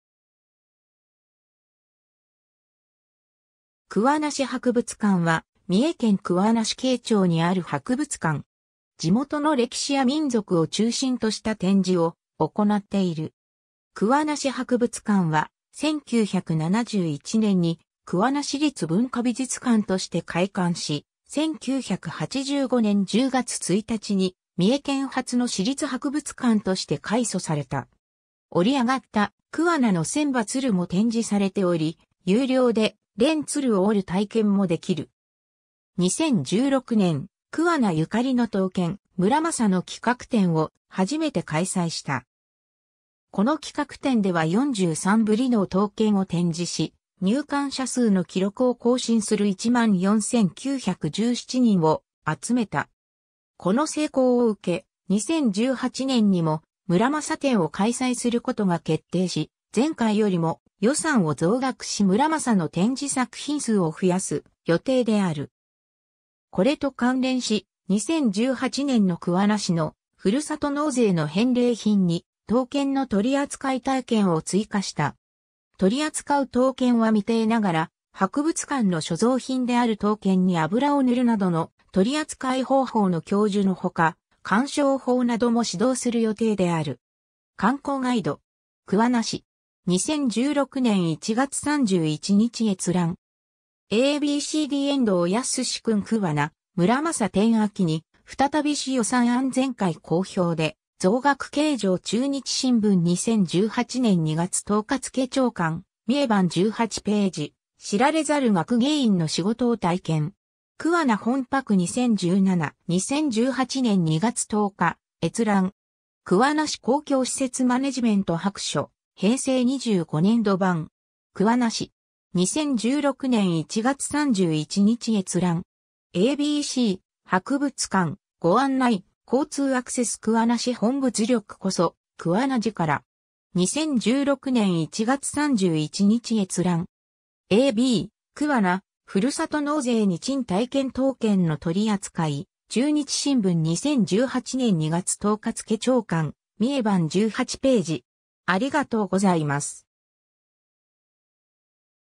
桑名市博物館
桑名市博物館（くわなしはくぶつかん）は、三重県桑名市京町にある博物館。
地元の歴史や民俗を中心とした展示を行っている。
 桑名市博物館は、1971年（昭和46年）に桑名市立文化美術館として開館し、1985年（昭和60年）10月1日に三重県初の市立博物館として改組された。
 折り上がった「桑名の千羽鶴」も展示されており、有料で連鶴（千羽鶴）を折る体験もできる。
 2016年（平成28年）、桑名ゆかりの刀剣・村正の企画展を初めて開催した。
この企画展では43振（うち半数が村正と関係作品）の刀剣を展示し、入館者数の記録を更新する14,917人を集めた。
この成功を受け、2018年（平成30年）にも村正展を開催することが決定し、前回よりも予算を増額し村正の展示作品数を増やす予定である。
 これと関連し、2018年（平成30年）の桑名市のふるさと納税の返礼品に「刀剣の取り扱い体験」を追加した。
取り扱う刀剣は未定ながら、博物館の所蔵品である刀剣に油を塗るなどの取り扱い方法の教授のほか、鑑賞法なども指導する予定である。
 ^ “観光ガイド (PDF)”. 桑名市. 2016年1月31日閲覧。
 ^ a b c d 遠藤康訓"桑名「村正」展 秋に再び 市予算案 前回好評で増額計上"中日新聞2018年2月10日付朝刊、三重版18ページ ^ “知られざる学芸員の仕事を体験!”. 桑名ほんぱく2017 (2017年). 2018年2月10日閲覧。
 ^ “桑名市 公共施設マネジメント白書 【平成25年度版】 (PDF)”. 桑名市. 2016年1月31日閲覧。
 ^ a b c 【博物館】ご案内・交通アクセス - 桑名市～本物力こそ、桑名力。
 ^ a b 「桑名・ふるさと納税に珍体験 刀剣の取り扱い」中日新聞2018年2月10日付朝刊、三重版18ページ